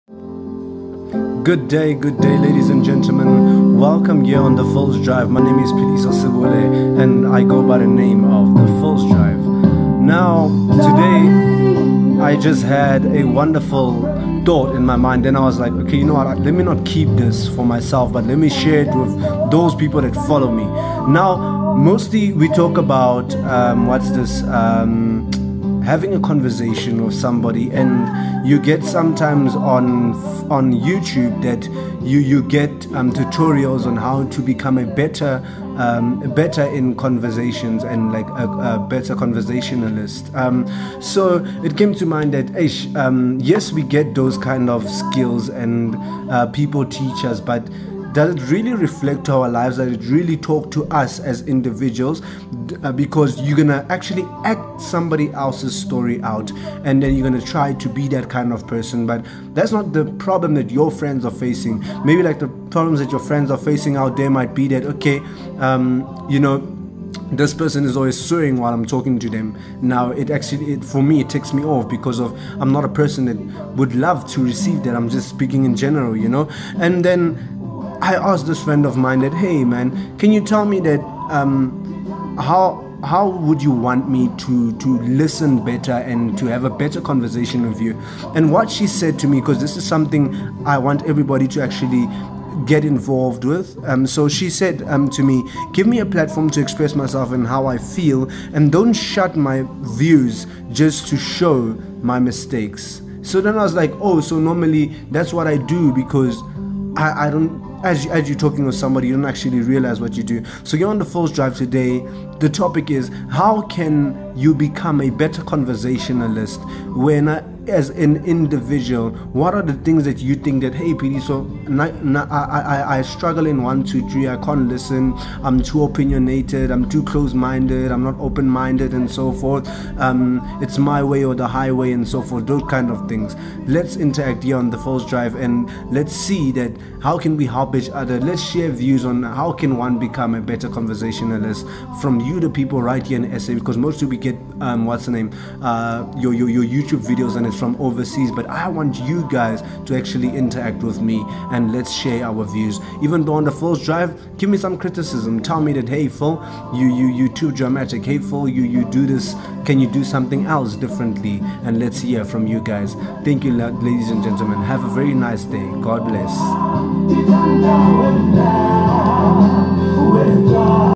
How to become a better conversationalist| PhilsDrive|South African Youtuber🇿🇦
At times we struggle on how to listen or even speak to our loved ones, strangers or even just life in general. Make me become a better person as well. Let's help one another On The PhilsDrive.